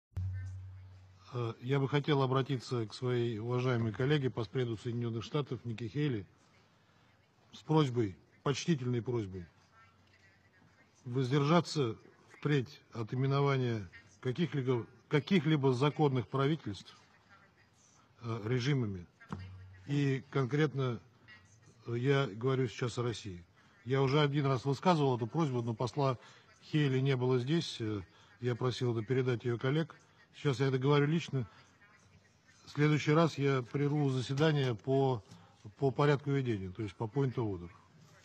Постпред РФ в ООН Небензя попросил не называть режимом власть в России: "Я прерву заседание"
Во время заседания Совета Безопасности ООН по вопросу Сирии постпред РФ Василий Небензя попросил представителя от США Никки Хейли не называть Россию режимом. "Я бы хотел обратиться к своей уважаемой коллеге, постпреду США Никки Хейли с почтительной просьбой воздержаться впредь от именования каких-либо законных правительств режимами. И конкретно я говорю сейчас о России. Я уже один раз высказывал эту просьбу, но посла Хейли не было здесь, я просил это передать ее коллег. Сейчас я говорю это лично. В следующий раз я прерву заседание по порядку ведения", - заявил он. Источник